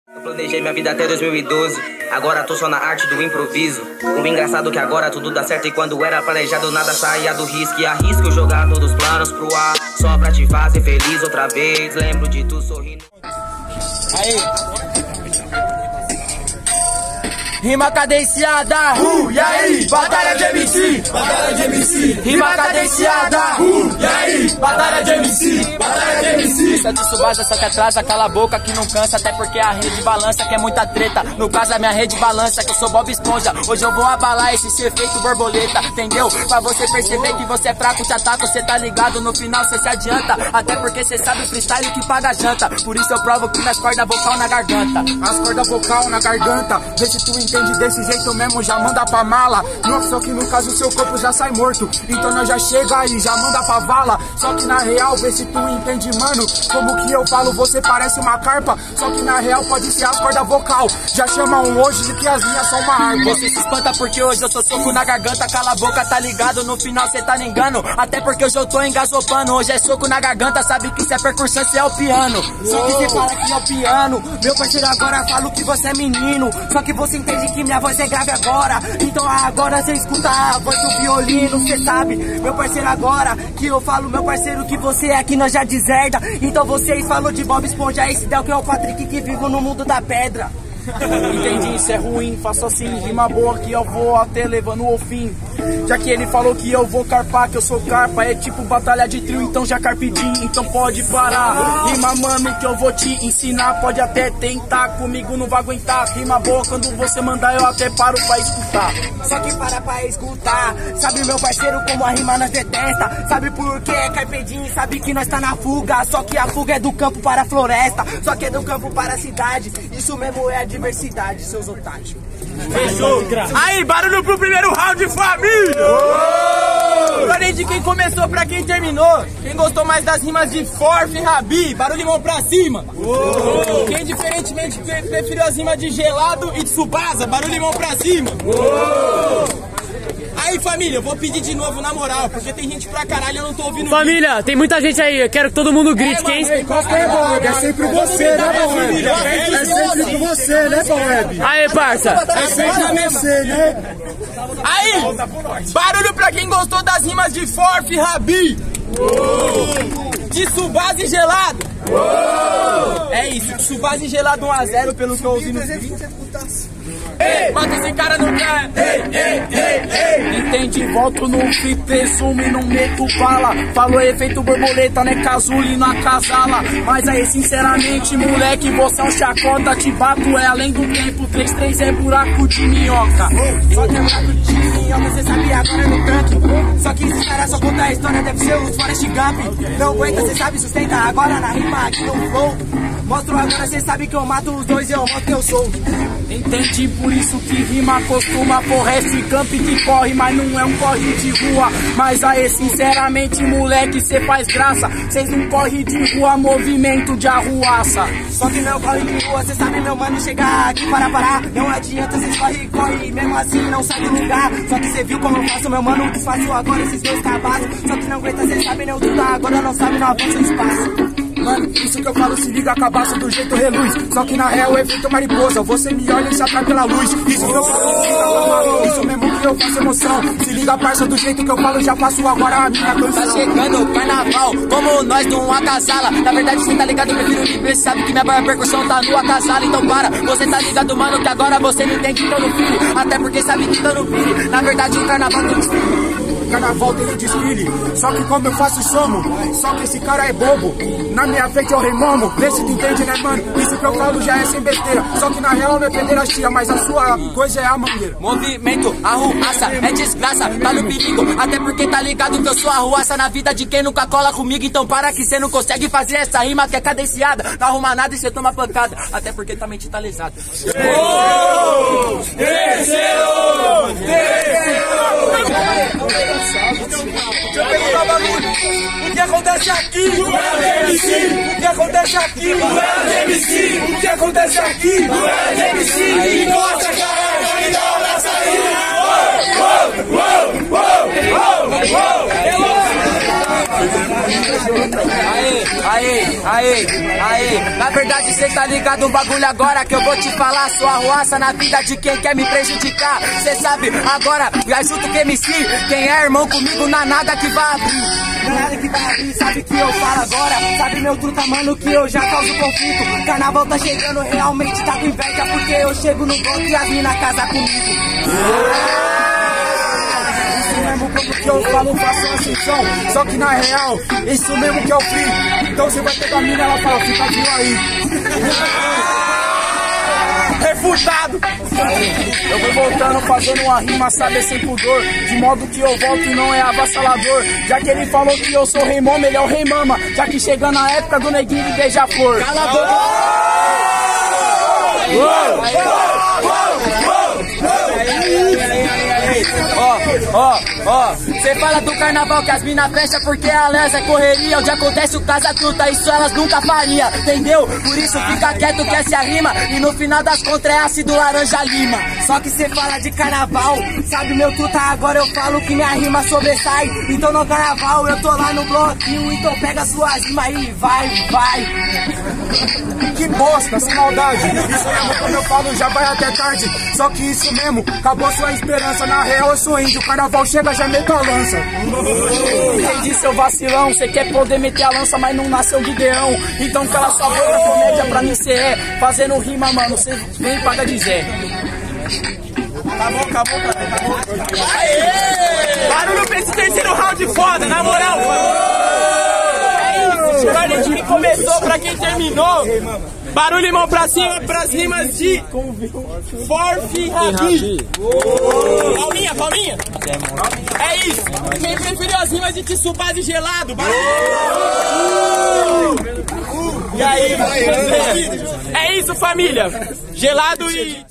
Gelado e Tsubasa x Forfe e Habi | 1ª Batalha da Norte | Santana | SP
A Batalha da Norte acontece todas as sextas, na Praça Margarida de Albuquerque Gimenez, ao lado da SPTrans Santana, às 19:30.